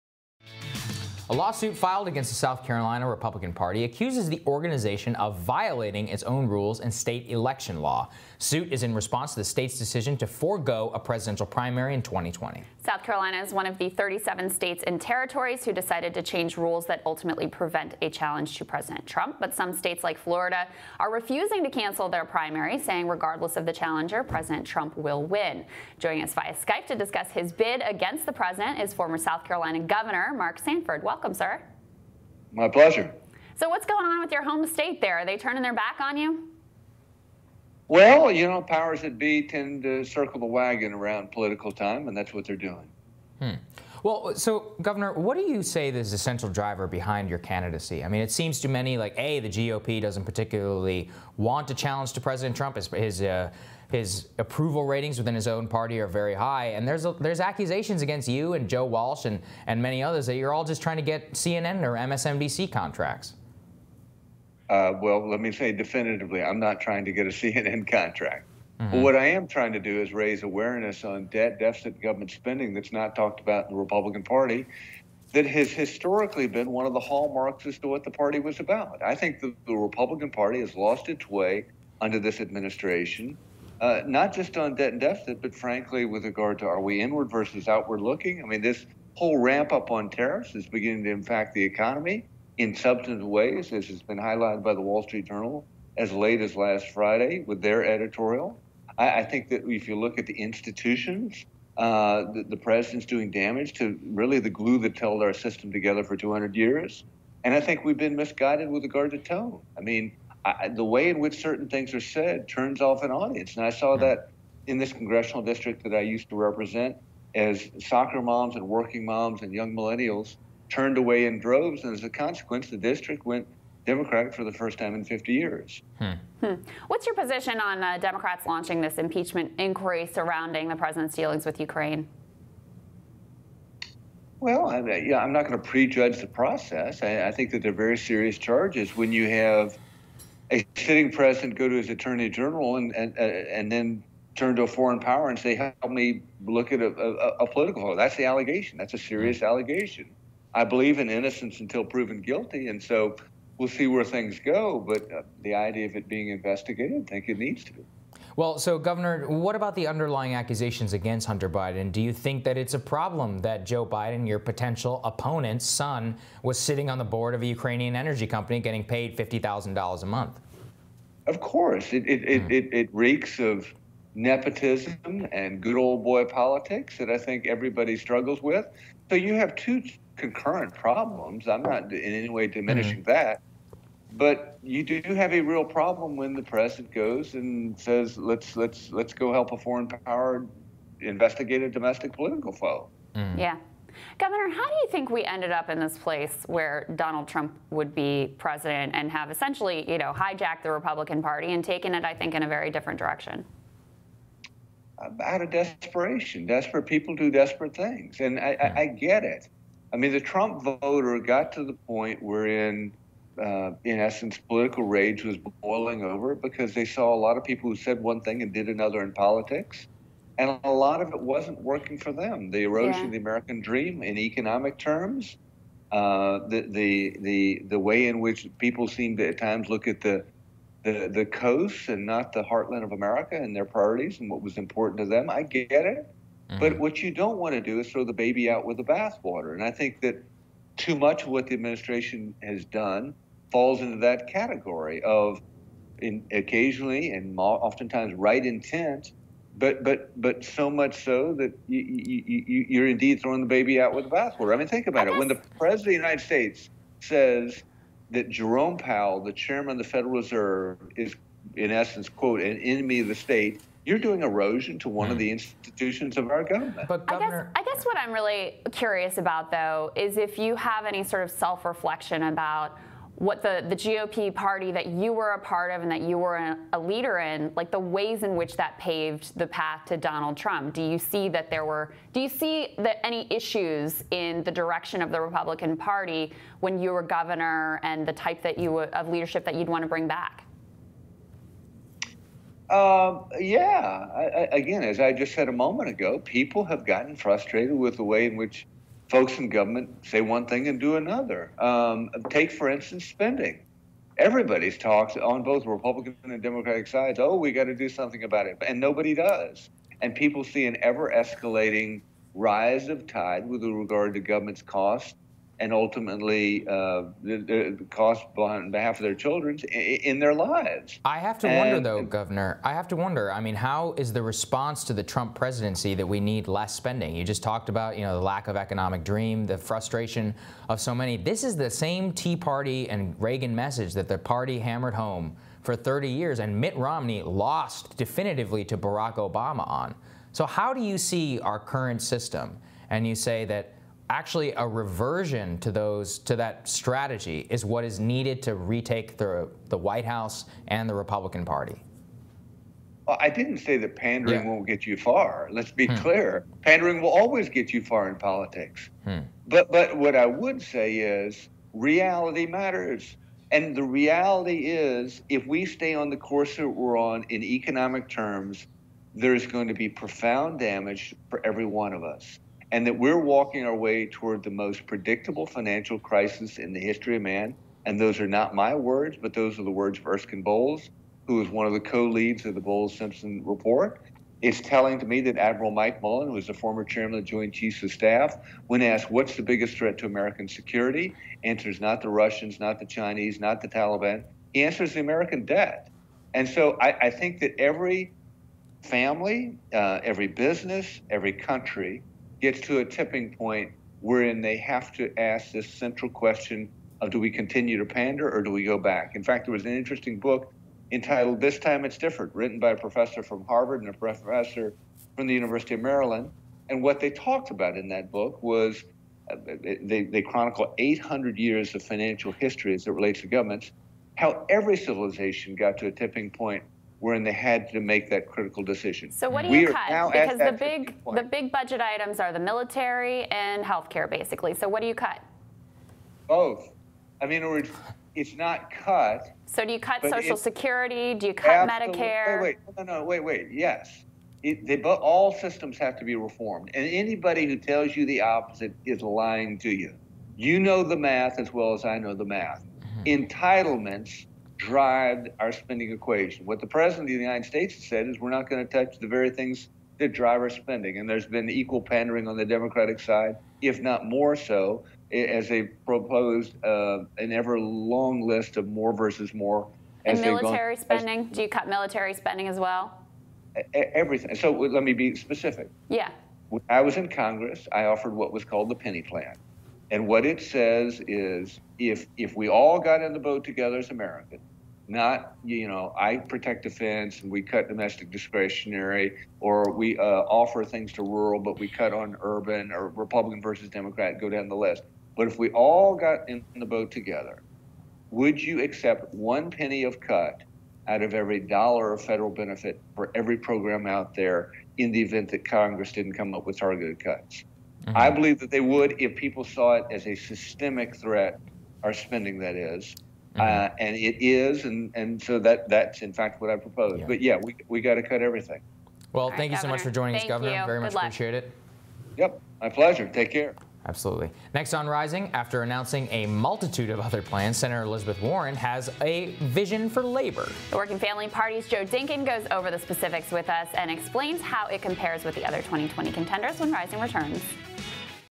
Gov. Mark Sanford: The Biden-Ukrainegate 'reek of good ole boy politics'
Republican Primary Candidate Fmr. Governor Mark Sanford makes his case for the presidency.